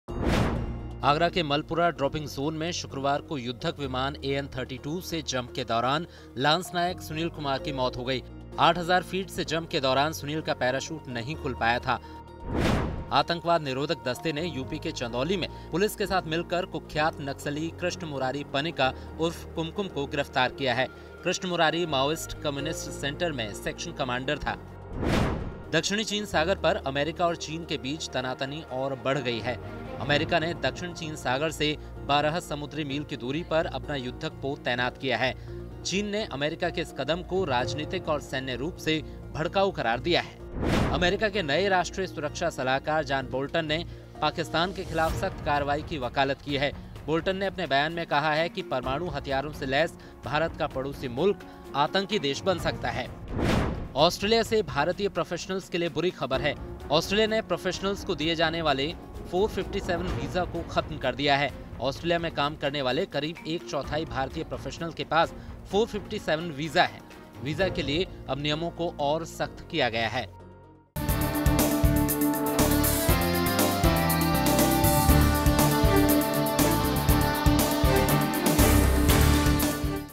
अभी तक की पांच बड़ी खबरें | Bharat Tak
1-
आगरा के मलपुरा ड्रॉपिंग जोन में शुक्रवार को युद्धक विमान एएन-32 से जंप के दौरान लांस नायक सुनील कुमार की मौत हो गई। आठ हजार फीट से जंप के दौरान सुनील का पैराशूट नहीं खुल पाया था।
2-
आतंकवाद निरोधक दस्ते ने यूपी के चंदौली में पुलिस के साथ मिलकर कुख्यात नक्सली कृष्ण मुरारी पनिका उर्फ कुमकुम को गिरफ्तार किया है। कृष्ण मुरारी माओइस्ट कम्युनिस्ट सेंटर में सेक्शन कमांडर था।
3-
दक्षिणी चीन सागर पर अमेरिका और चीन के बीच तनातनी और बढ़ गई है। अमेरिका ने दक्षिण चीन सागर से 12 समुद्री मील की दूरी पर अपना युद्धक पोत तैनात किया गया है। चीनी ने अमेरिका के इस कदम को राजनीतिक और सैन्य रूप से भड़काउ करार दिया है।
4-
अमेरिका के नए राष्ट्रीय सुरक्षा सलाहकार जान बोल्टन ने पाकिस्तान के खिलाफ सख्त कार्रवाई की वकालत की है। बोल्टन ने अपने बयान में कहा कि परमाणु हथियारों से लैस भारत का पड़ोसी मुल्क आतंकी देश बन सकता है।
5-
ऑस्ट्रेलिया से भारतीय प्रोफेशनल्स के लिए बुरी खबर है। ऑस्ट्रेलिया ने पेशवरों को दिए जानेवाले 457-वीजा को खत्म कर दिया है। ऑस्ट्रेलिया में काम करनेवाले करीब एक चौथाई भारतीय प्रोफेशनल्स के पास 457-वीजा है। वीजा के लिए अब नियमों को और सख्त किया गया है।